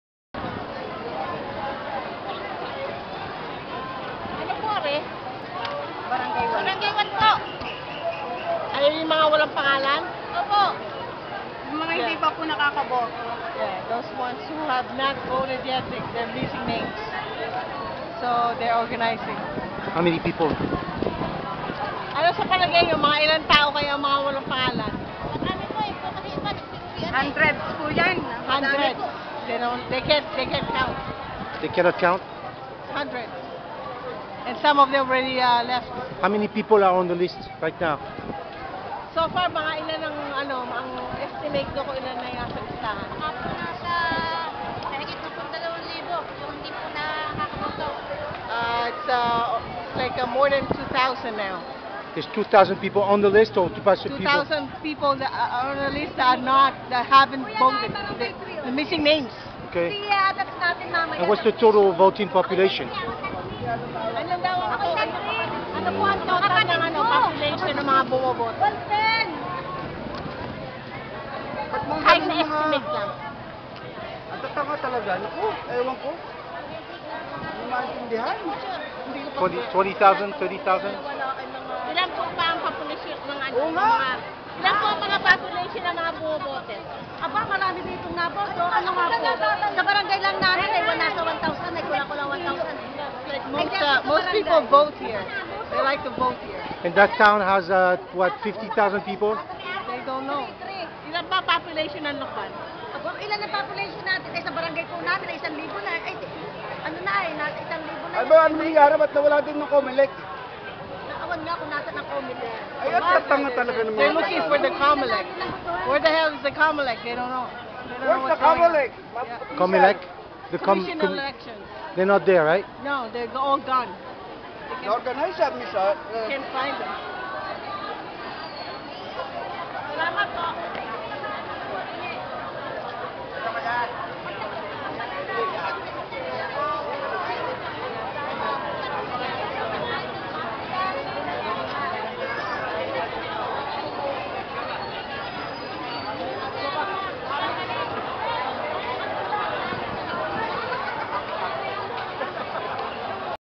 Volunteers are assisting thousands of disenfranchised voters in the town of Lucban Quezon, Philippines, where a mayor with an environmental vision has kept corporations and polluters in check. In a town which the 2010 census says has 46,700 residents, on May 9th, 2016, election day at 11 AM, already 2,000 plus voters have signed the complaint list because they were not allowed to vote. All voting machines are broken, lines at the local university are 300 plus long and the election commission is nowhere to be found. While voters are on the election lists, hundreds are being denied the right to vote and are asked to vote on a piece of paper and hand it out to the officials. Many voters refuse. Tension is high and police forces are in the background.